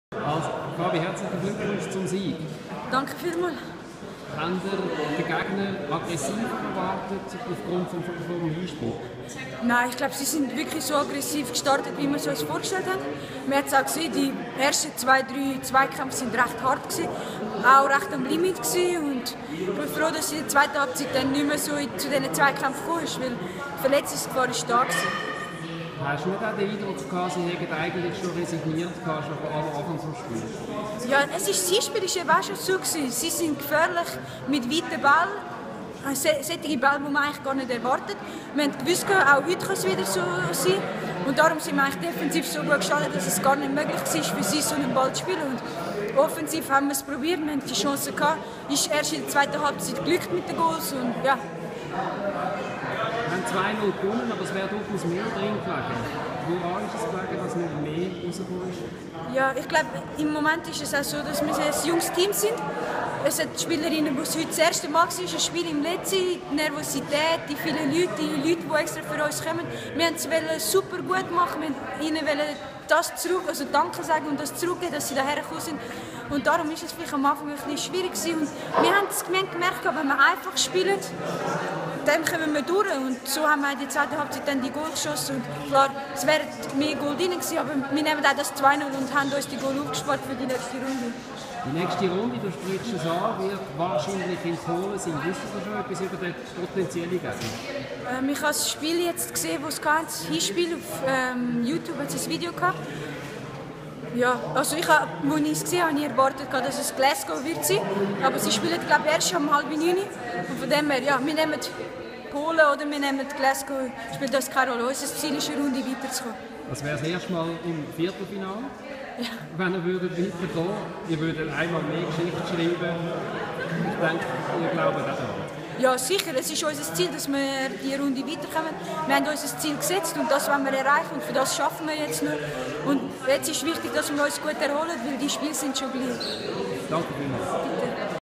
FCZ-Captain Fabienne Humm zum Sieg in der Champions League
Nach dem Sieg gegen Osijek zieht Humm Bilanz und blickt auf die nächste(n) Runde(n)